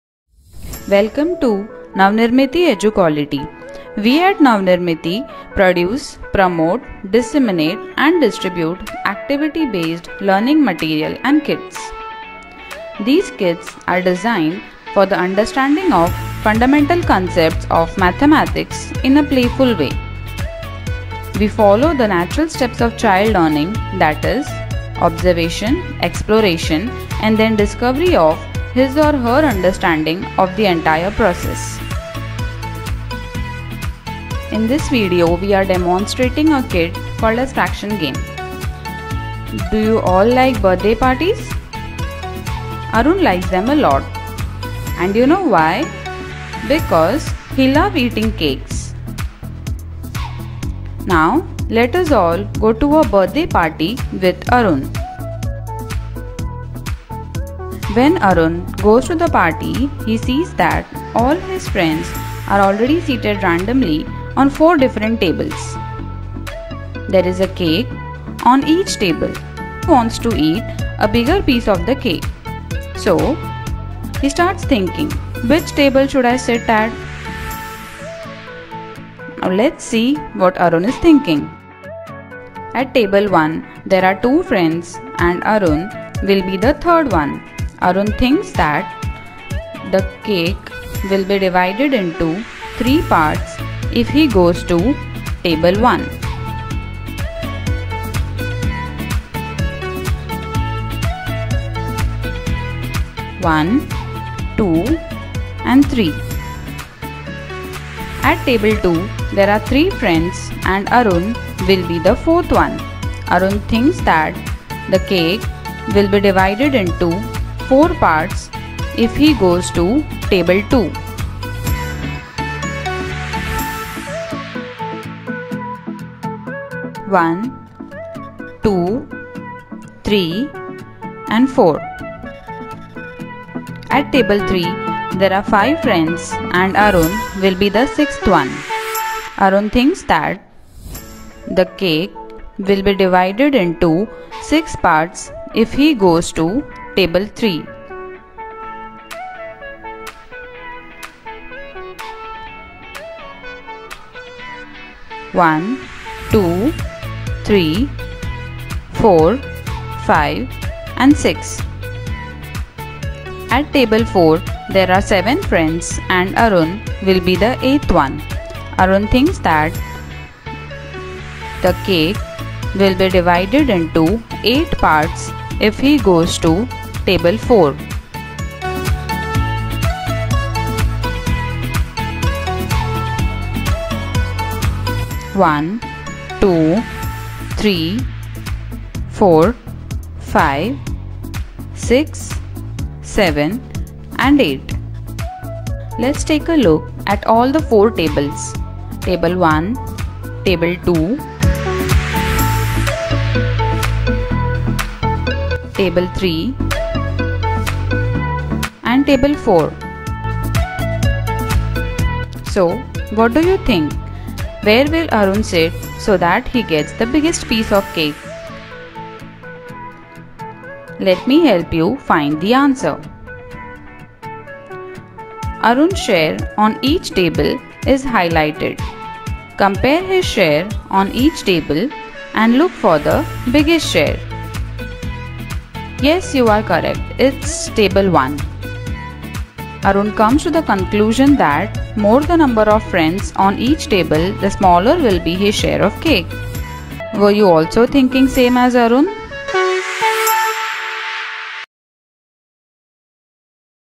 Introducing Fractions Through Fraction Game Kit
Introducing the basic concept that fractions are part of a whole and also indirectly comparing fractions is done using the fraction game kit. Comparing is one of the major difficulties in fractions. The basic understanding of more the number of pieces the whole is divided into, smaller will be each person’s share is conveyed through the story of the birthday party.